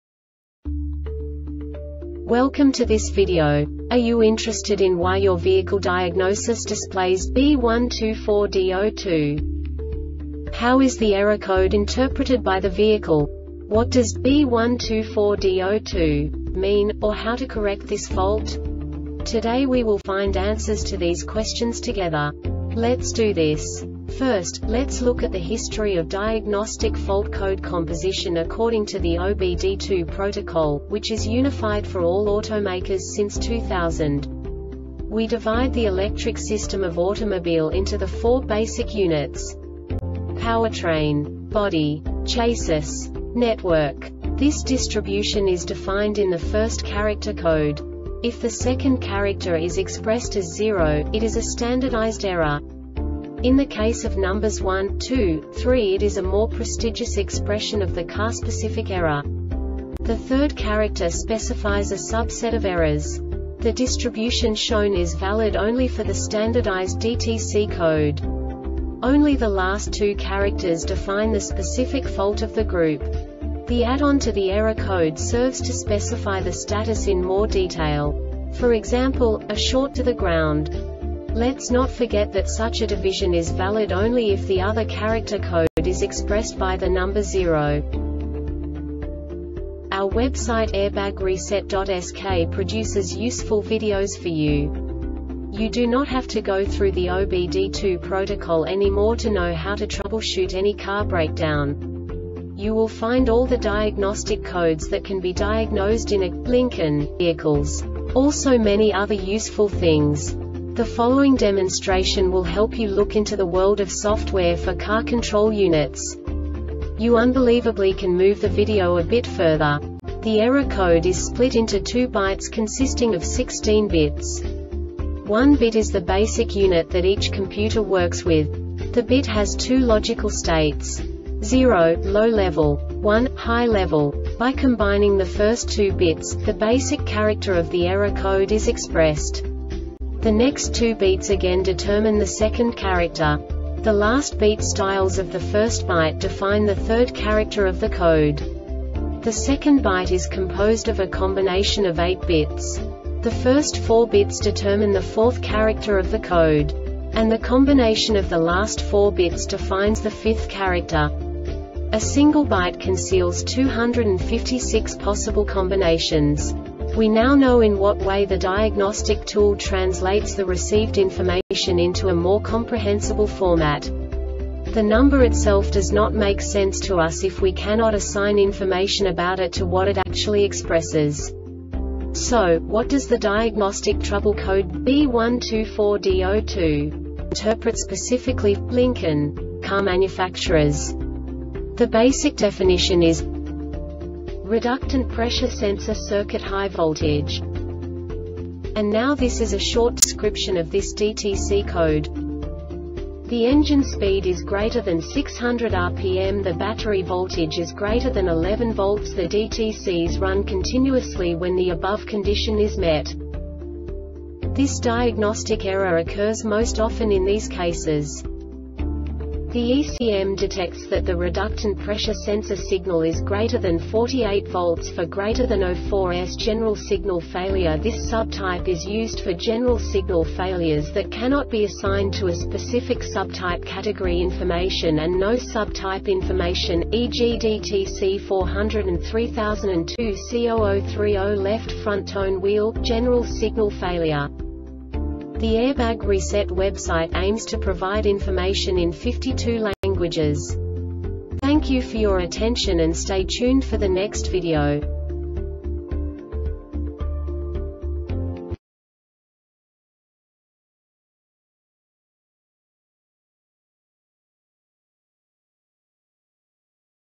DTC Lincoln B124D-02 Short Explanation
Contents:
0:21 Basic DTC analysis according to OBD2 protocol standard.
1:48 Insight into programming
2:58 A diagnostically understandable description of the Diagnostic Trouble Code
3:05 Basic error definition

Make:
Lincoln
Code:
B124D 02
Definition:
Reductant Pressure Sensor Circuit High Voltage
Description:
The engine speed is greater than 600 RPM.The battery voltage is greater than 11 V.The DTCs run continuously when the above condition is met.
Cause:
The ECM detects that the reductant pressure sensor signal is greater than 4.8 V for greater than 0.4 s.